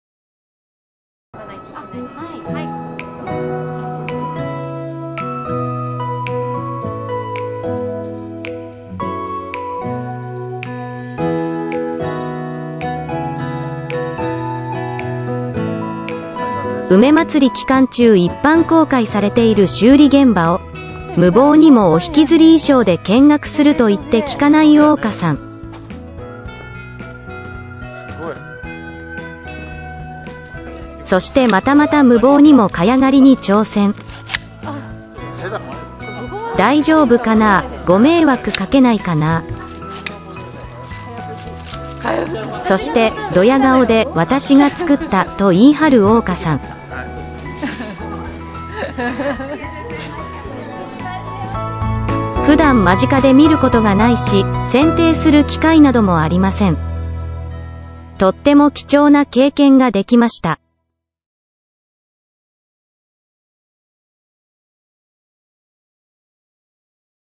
Cosplayer Ouka桜花梅まつりイベント紹介 Vol.1 古民家修理現場見学
奈良県大和民俗公園、奈良県立民俗博物館で開催された2024「梅まつり」。
コスプレイヤー桜花のイベント紹介。古民家の修復現場見学です。これは修理工事期間中で、しかも、イベントで一般公開している時にしか体験できないもので、大変貴重な経験をしました。
さとやまでもあまり見かけなくなった茅葺屋根。しかもそれを間近で見て、触って、そして剪定体験まで経験しました。
ついつい「どや顔」になってしまいます。